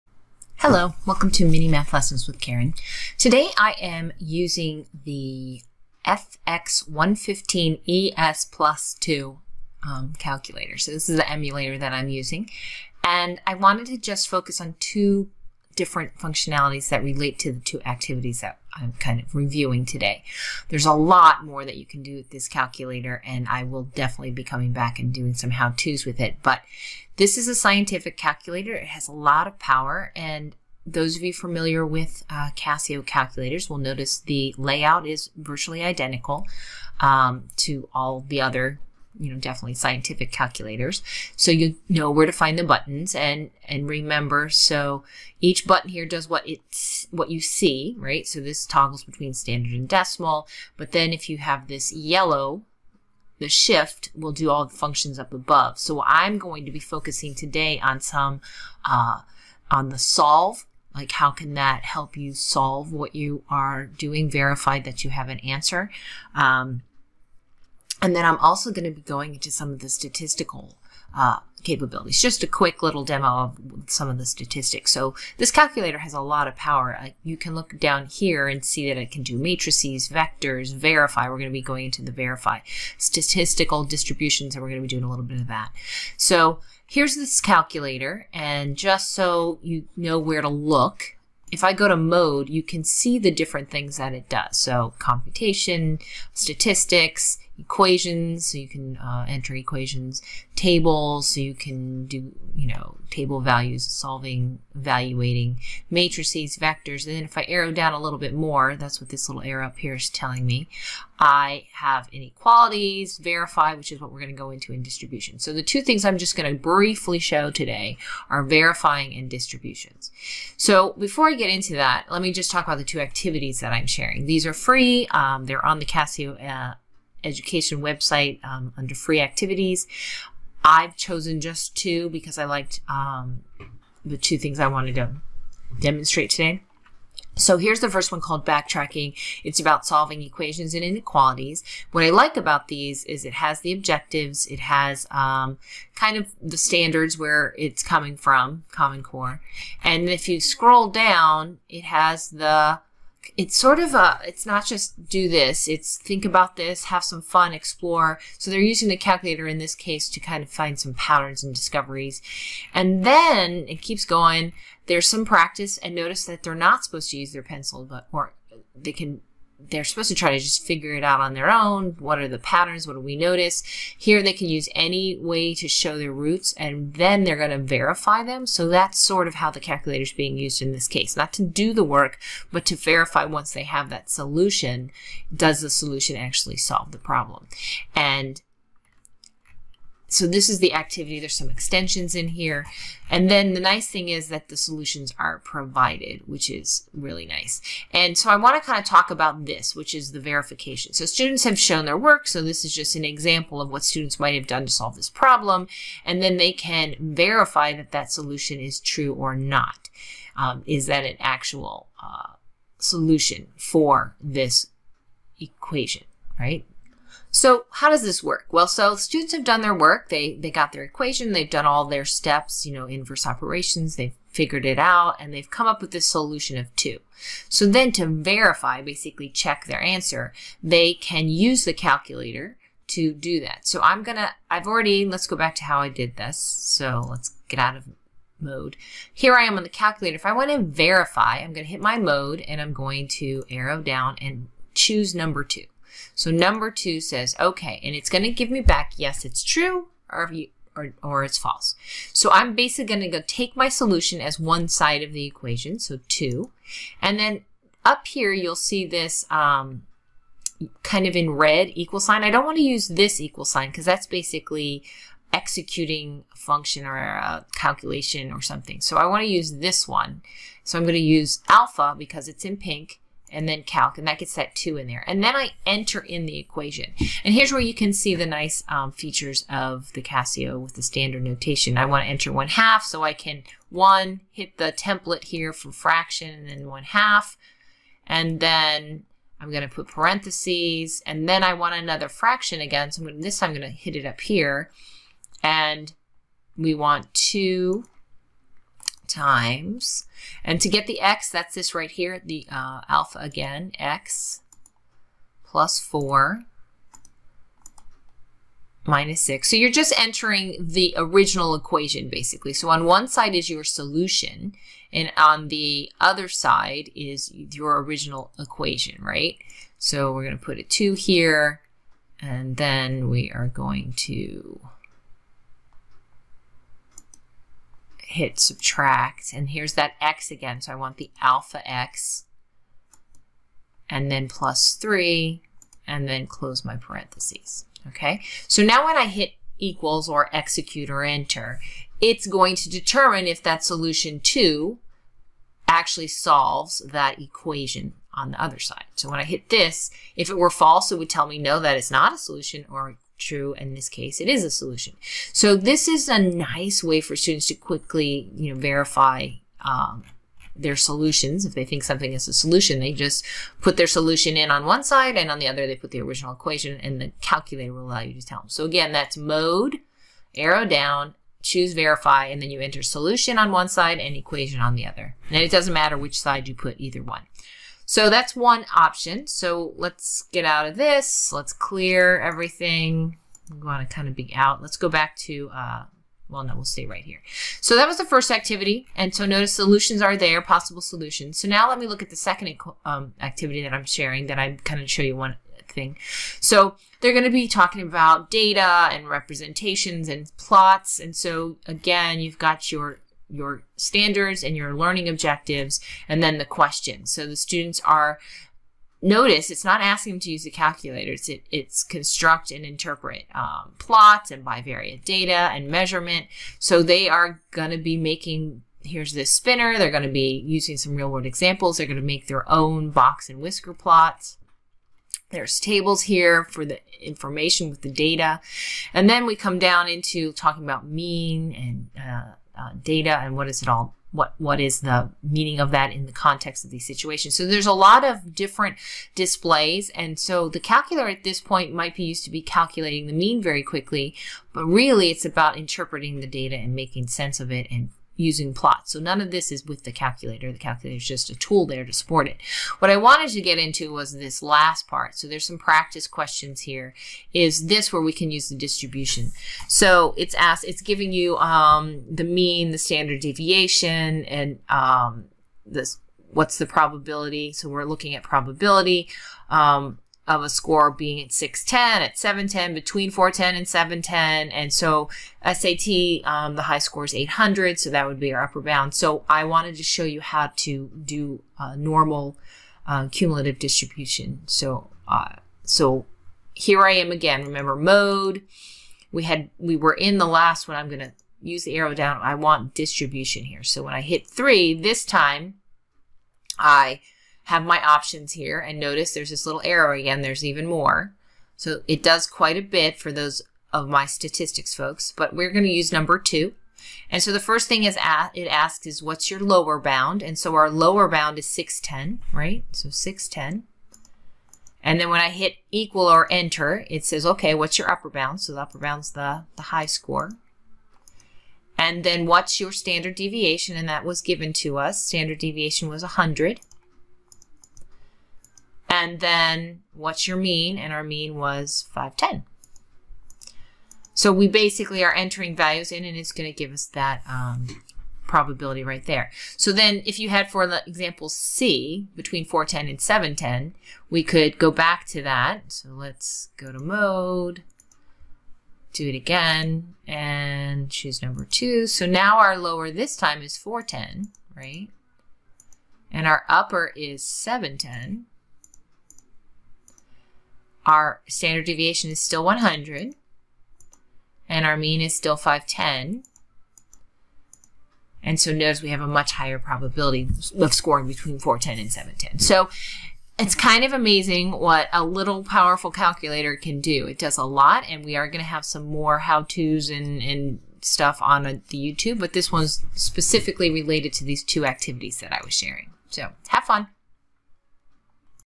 fx-115ES+2: Verify Solutions and Normal Curve Probability (Mini-math Lesson w/Karen Greenhaus)
This video shows how to verify solutions to equations on the Casio fx-115ESplus2 scientific calculator. It also shows how to find the probability in a normal curve distribution. There are two free activities that are previewed in this video where these two functionalities are needed. A link to the free activities is included below.

BackPacking (Solving Equations) - (pgs 11-16)

Describe and Infer (data and distributions) (pgs 27-36)